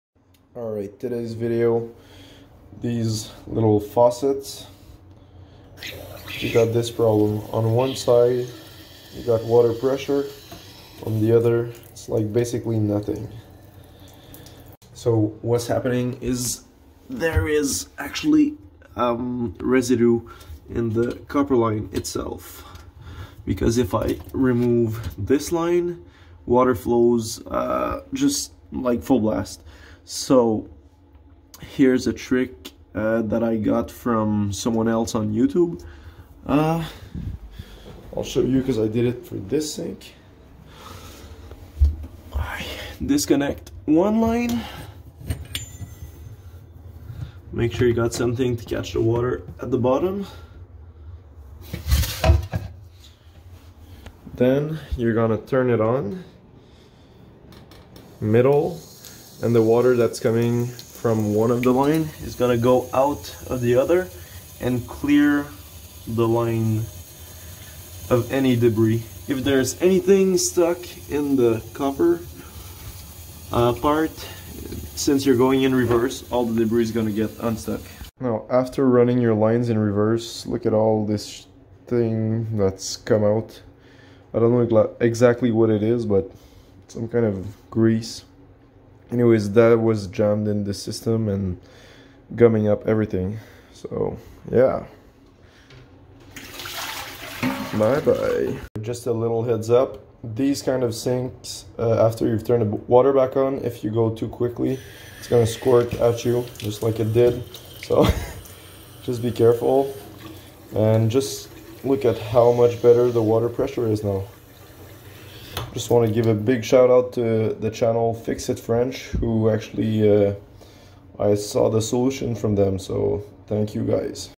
Low water pressure/clogged line bathroom faucet
This is how i fixed both my bathroom faucets, which had basically no water pressure on the hot water line.

Full disclosure i'm not a plumber, this is just a DIY video, hope it helps you out.